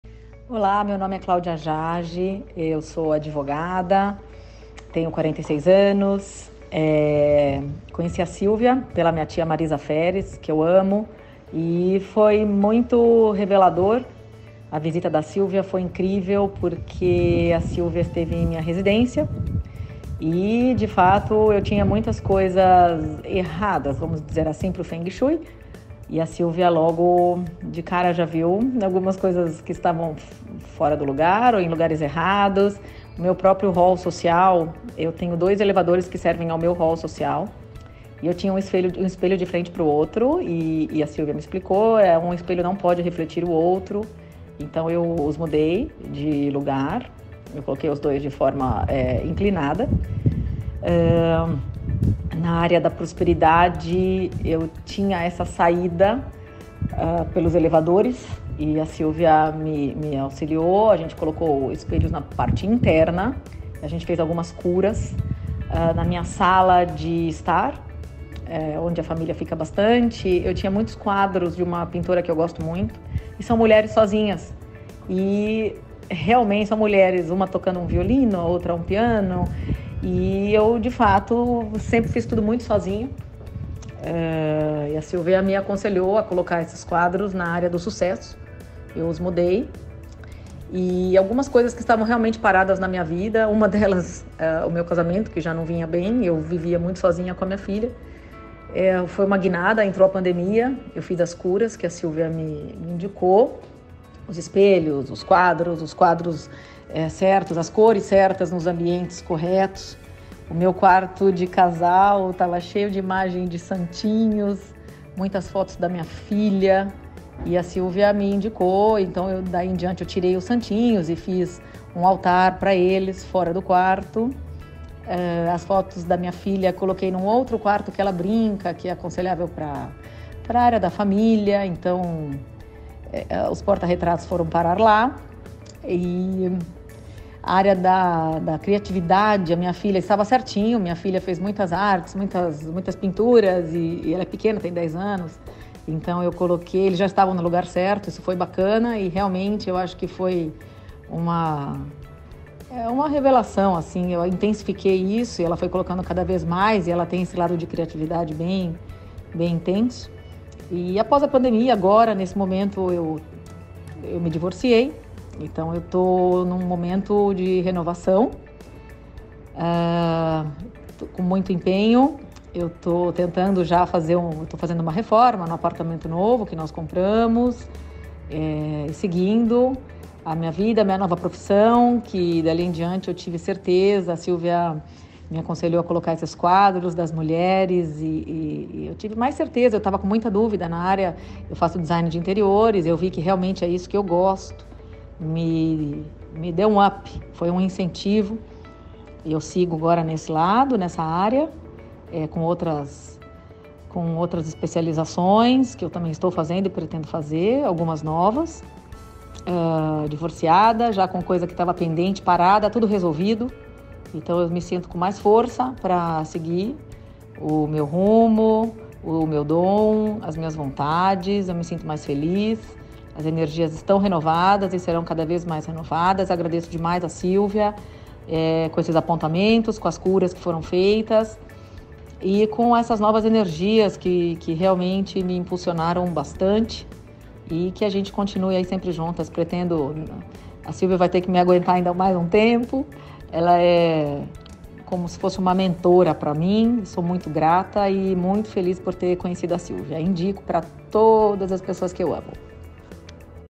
Depoimento CLAUDIA AJAJ...LIVRO 2 GOOD VIBES For You! CASAS,CASOS E CURAS..
Quando resolvi fazer Feng Shui, em princípio, queria verificar o que estava errado em termos de energia na minha casa. Mandei as plantas para Silvia Cesar dentro de uma pasta vermelha, como solicitara.Depois de uns dias, ela apareceu em casa com tudo já estudado: dividiu as plantas em setores e me explicou direitinho como funcionava área por área. Paciente (eu fazia muitas perguntas), passou um tempão comigo, mostrou o que deveria ser feito em cada espaço e ainda me deu o livro dela, Good Vibes For You, de presente. Gosto de saber para fazer, não só executar. Adoro aprender coisas novas.Na verdade, eu não tinha nenhum problema em foco. Queria que ela visse se havia alguma coisa errada na minha casa para eu corrigir, em todos os sentidos: família, trabalho, relacionamentos, sucesso, enfim, tudo. Queria que ela desse um geralzão. Eu acredito nas curas energéticas.Assim que entrou no apartamento, já reparou que os dois elevadores que servem meu hall social estavam bem na área do trabalho. Ela explicou que o movimento de sobe e desce dos elevadores prejudicaria as atividades do meu marido, pois levam a energia embora. Precisava colocar um espelhão no corredor e mais dois do lado da porta de serviço, para resolver esse aspecto.Disse também que eu tinha que fazer algumas curas na casa de um modo geral.Quando comecei a fazer o Feng Shui, meu marido, que não gostava muito de espelhos, estranhou. Mas não teve opções, pois fui bastante convincente. Ele percebeu que eu estava levando muito a sério, então respeitou.Eu tinha uns quadros de mulheres solitárias tocando instrumentos (piano, violino, etc.). Silvia falou que a posição desses quadros estava completamente errada, porque incidia bem na área da família: “São mulheres solitárias, tira isso daí, coloca porta-retratos com fotos da sua família”.Minha filha de oito anos tinha feito uns quadrinhos que estavam no corredor. Silvia falou que ficou excelente ali, porque aquele corredor tinha a ver com a parte criativa. Tudo o que a minha filha quisesse fazer e colocar naquela parede era para eu deixar. “Joga tudo o que você não está usando aqui fora, faz uma limpa, organiza os armários”. De fato, ela ficou bem mais tranquila. Nunca imaginei que meu closet pudesse estar prejudicando o sono da minha filha. A cama dela estava debaixo de um lustre de cristal, isso não podia acontecer, tinha que estar fora, porque o cristal tem incidência energética que atrapalha o sono. Não se deve dormir embaixo de cristais. Troquei a cama dela de posição.Fiz mais essa cura.Eu tinha uma Bíblia, santinhos, fotos de família, um monte de coisas no meu quarto. Ela mandou arrancar tudo aquilo e colocar em um outro lugar. Disse que quarto não era altar.Depois que comecei a fazer as curas da casa, acho que, já no primeiro mês, minha vida mudou para melhor.Estava iniciando um curso de design de interiores – tive mais certeza ainda de que era aquilo que eu deveria fazer, porque eu ainda tinha dúvidas de qual nova profissão seguir. Mesmo sem estar formada, consegui trabalho, fiz algumas decorações. Mudei radicalmente após os quarenta.Eu nem foquei a questão do sucesso com ela, falei de outras coisas, mas o que mais despertou foi mesmo essa questão da carreira, do sucesso profissional.Depois da Silvia, ficou tudo equilibrado aqui em casa. O Feng Shui foi um impulso na minha nova carreira.